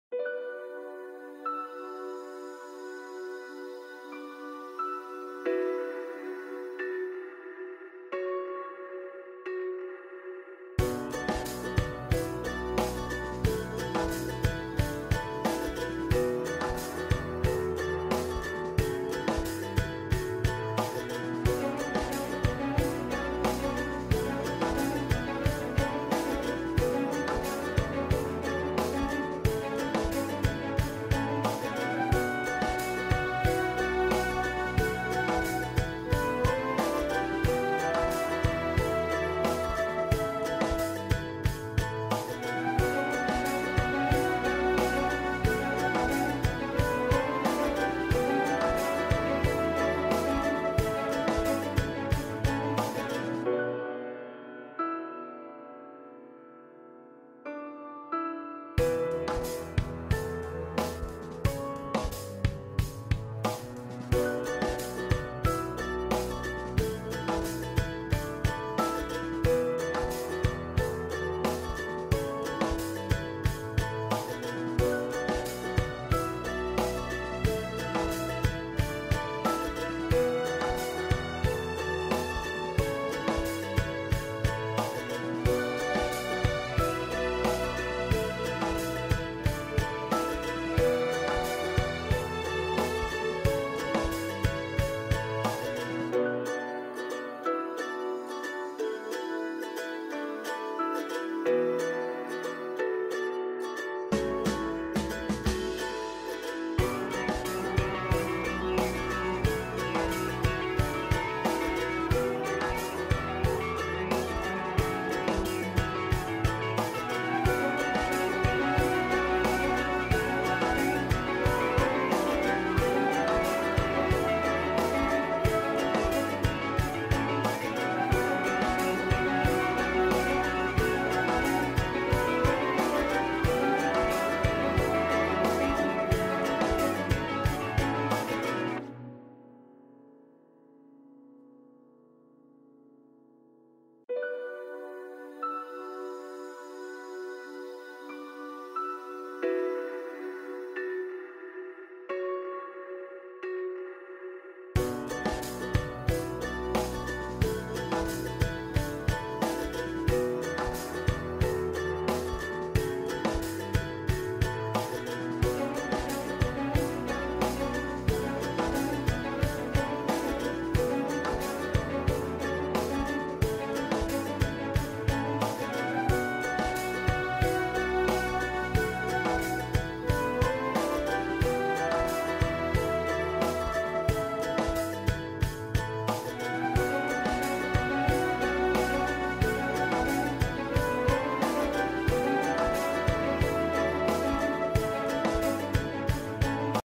3D Art - Beautiful and colourful 3D fevicryl outline flower design
This video show 3D art design that is beautiful and colourful 3D fevicryl outline flower design.This video shows how to learn these art  step by step  instructions in the easy way.Watch & Try it.